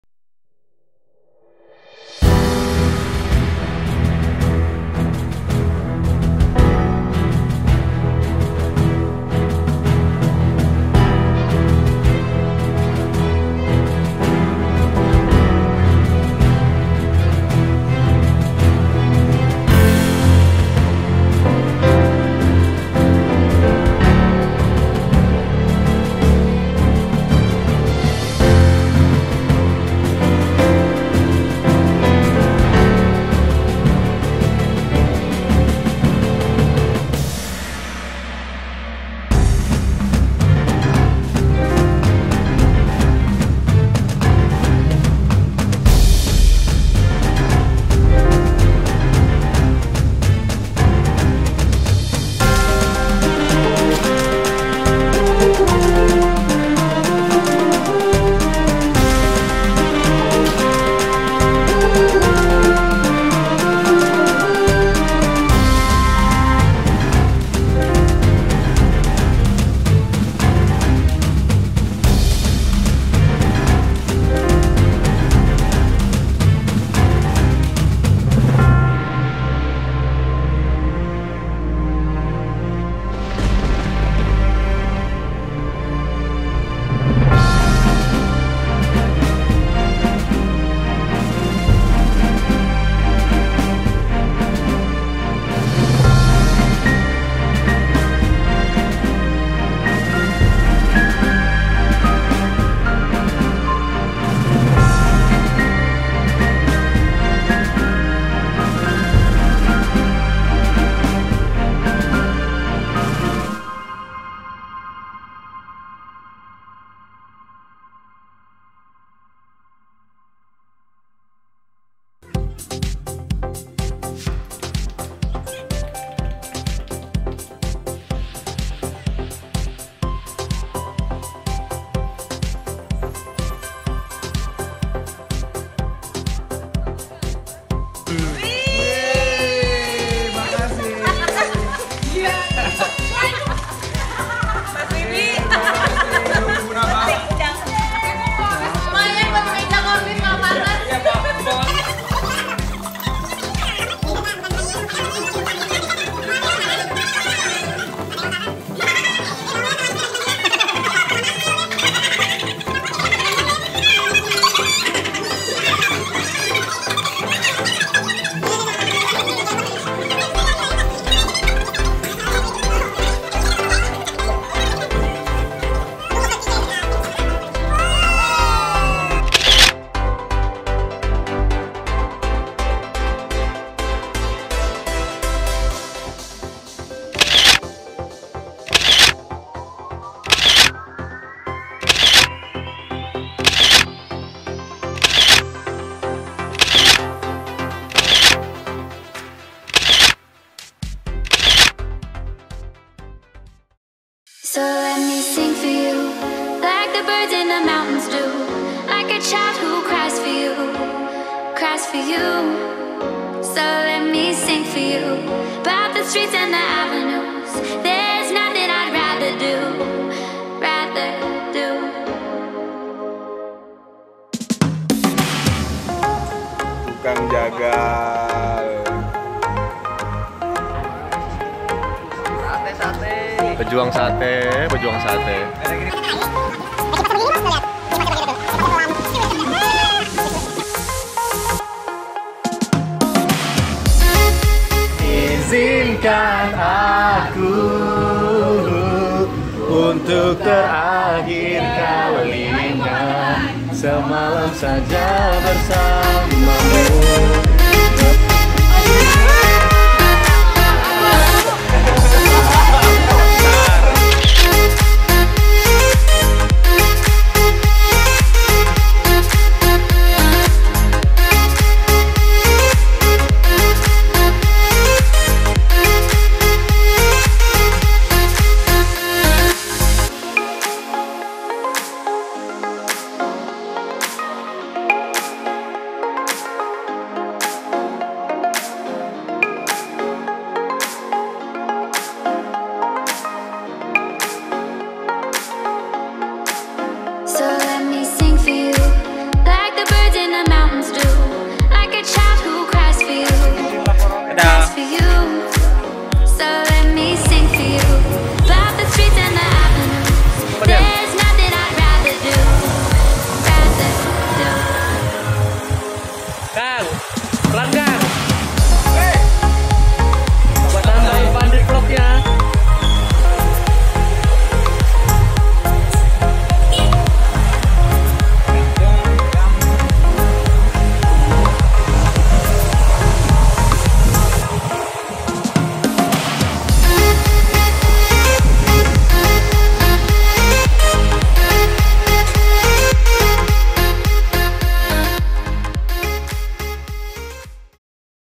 TRIP TO COHAUS Exclusive Villa (Metropolitan's Squad) Gathering!!!
Video ini merupakan dokumentasi saat Mini Family Gathering Desk Metropolitan MNC Media 27-28 April 2019. Sebagian besar pesertanya adalah Reporter dan Korlip